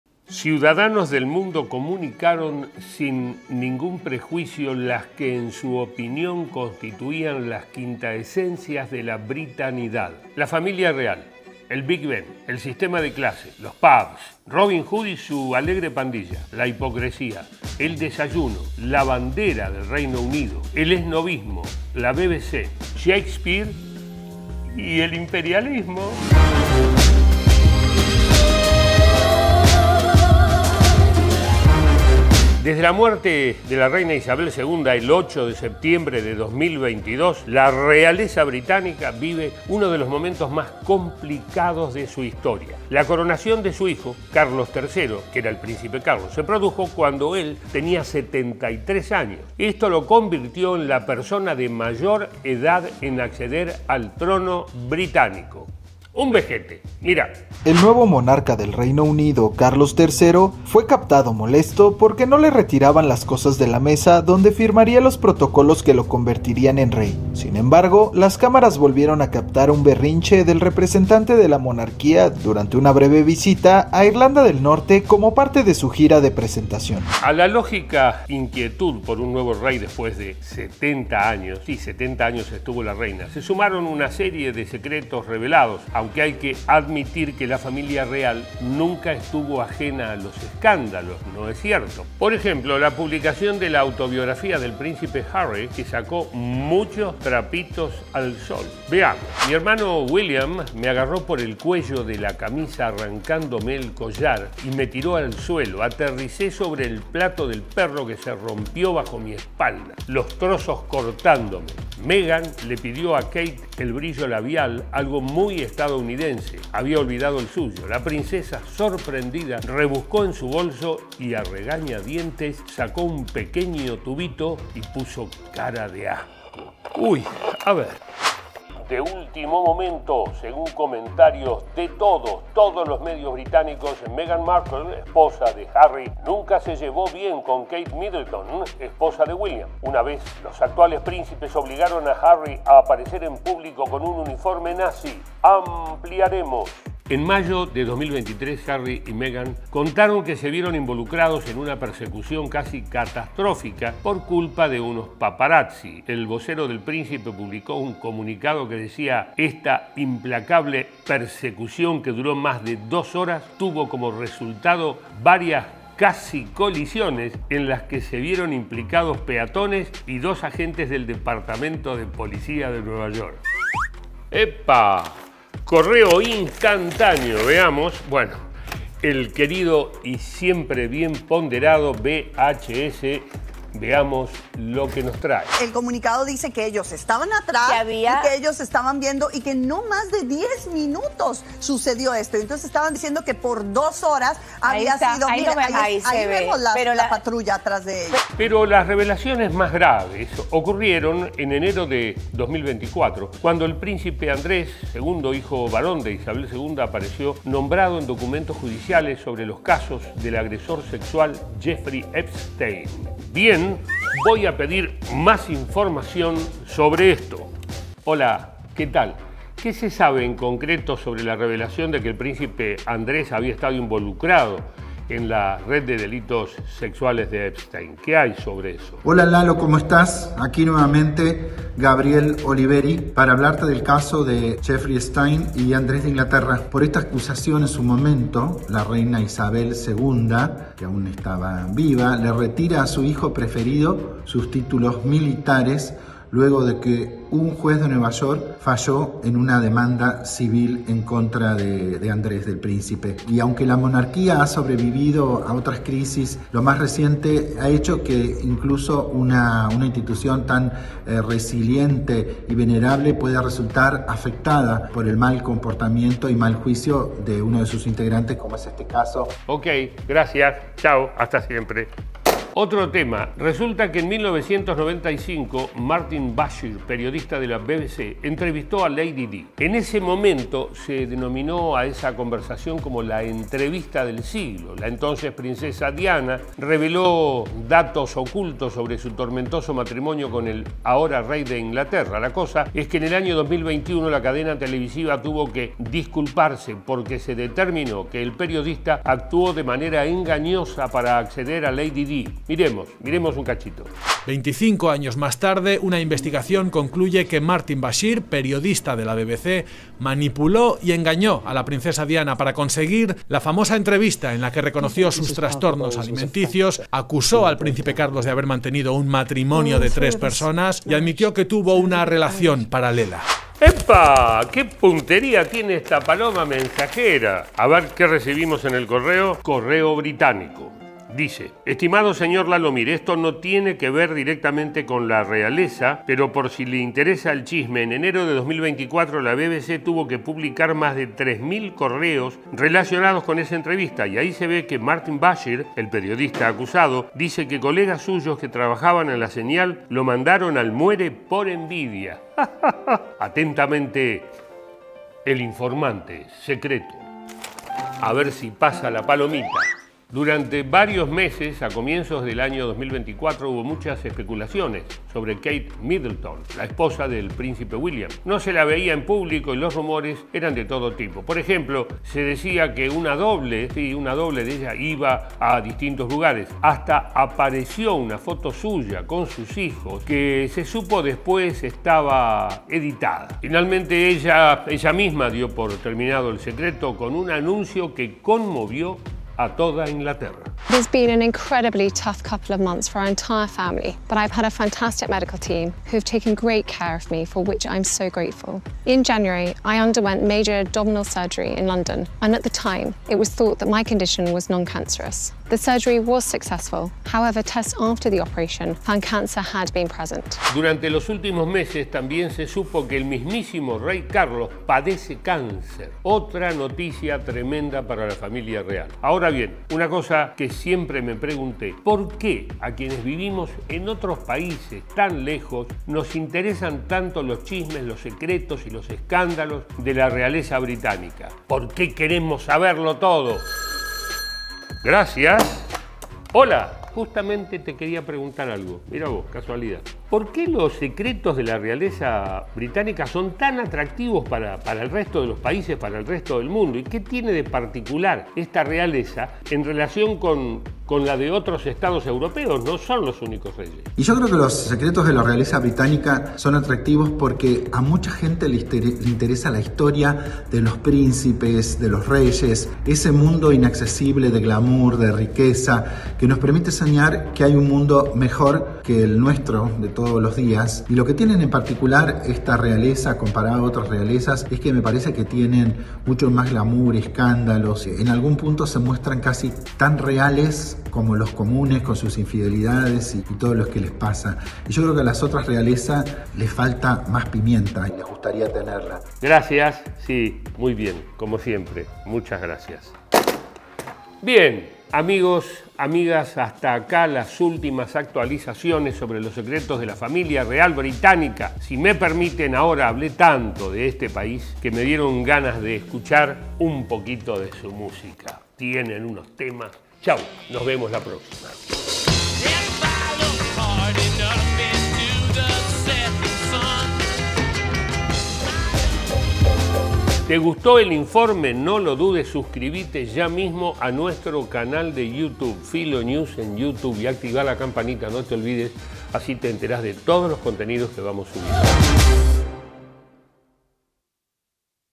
Kate MIDDLETON, Carlos III y los SECRETOS de la realeza británica sin revelar | Filo Explica
Filo Explica x Lalo Mir. Con la participación de Gabriel Oliveri

En la historia de la RealezaBritánica, la familia real nunca estuvo exenta de los secretos y escándalos, pero después de 70 años una nueva línea generacional trae con ella nuevas revelaciones.

Desde la destitución del príncipe Harry y su esposa Megan, hasta el fallecimiento de la princesa Diana, repasamos porque los secretos de la realeza repercuten en el resto del mundo.

Entérate de todos los detalles en este nuevo